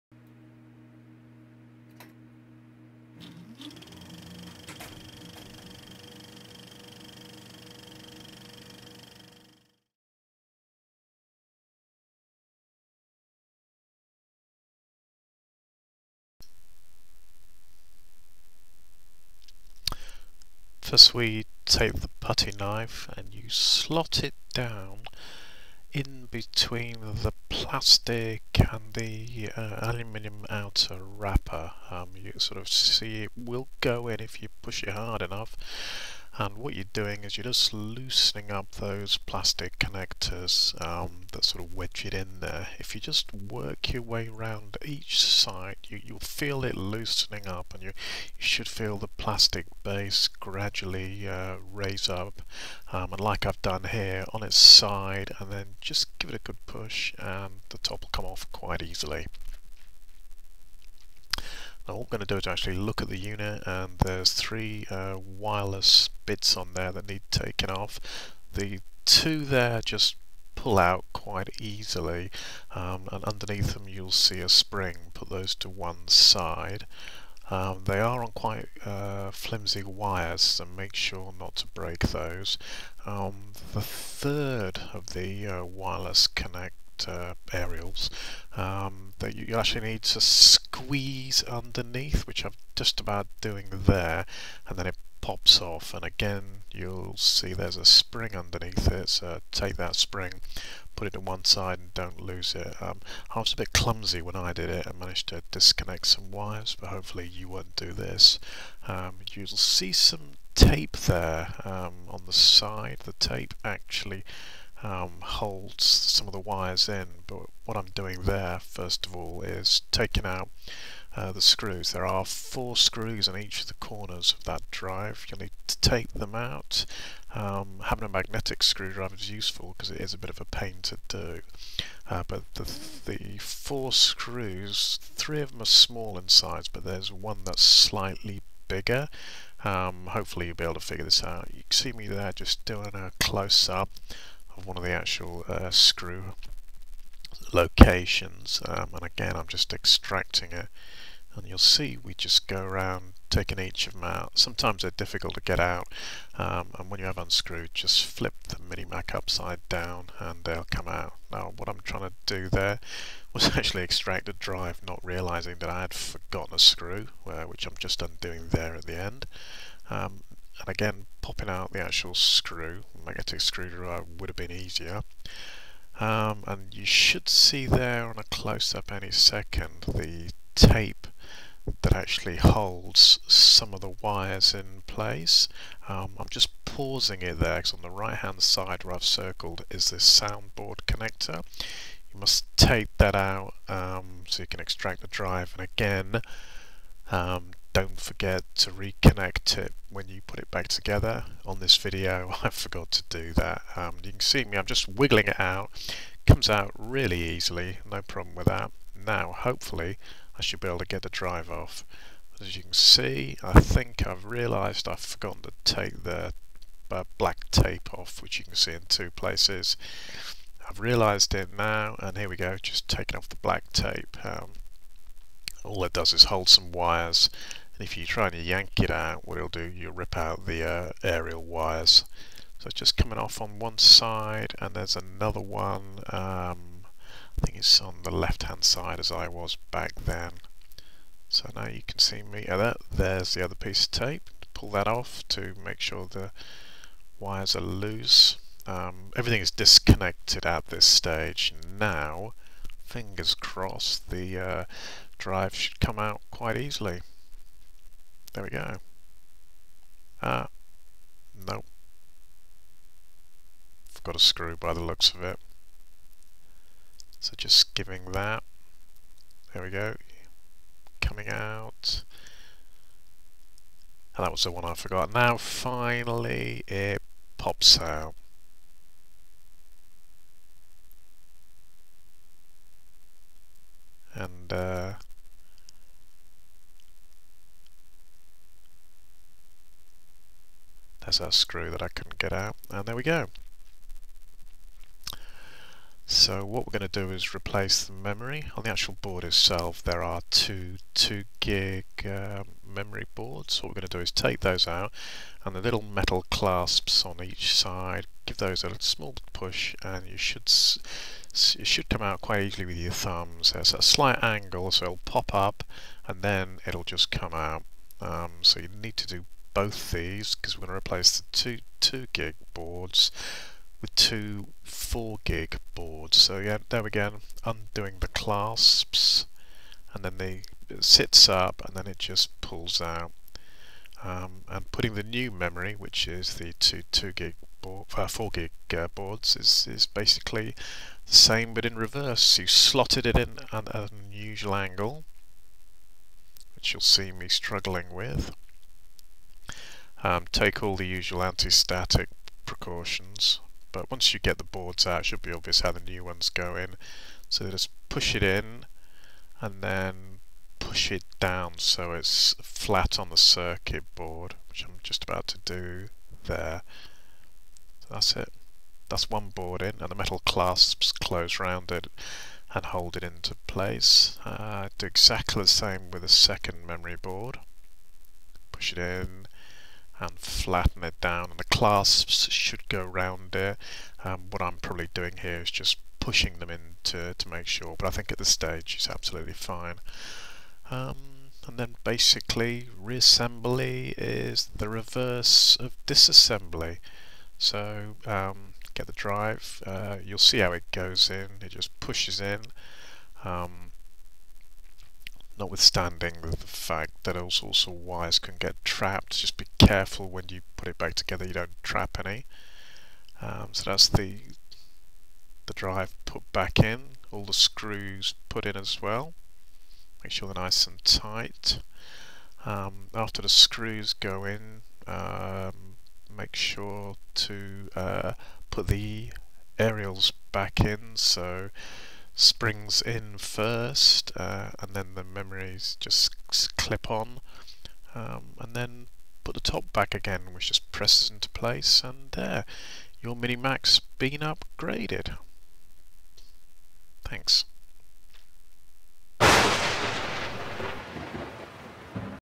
How to Upgrade your Apple Mac Mini RAM
A short instructional video showing how to take the Apple Mac Mini apart and replace the existing memory boards with larger capacity - converting your 4GB machine into an 8GB one.

This specifically applies to the Mac Mini 3.1 (Late 2009) model, but you'll find the same applies to other machines where Apple have not put in a 'service slot'.

The extra memory cost £59 from a local store - so it was a cost effective upgrade.

You don't need many tools, except a set of watchmaker screwdrivers and a putty knife - which is a flexible knife but with a blunt end.

Once the device is apart the drive is held on to the motherboard by 4 screws and you'll see me mess about thinking I've got them all out when there is still one left.

Be careful with the aerial wires, as these are delicate and one of mine came out - but they do plug back in okay.

Also don't forget to plug the sound board back in, as I forgot and spent the weekend trying to figure out why the OSX sound drivers were not loading as the machine booted up.

Guess if I'd watched my own video I'd have realised I forgot to reconnect !!

Anyway, with this you'l get at least another year your mac mini before the inevitable upgrade.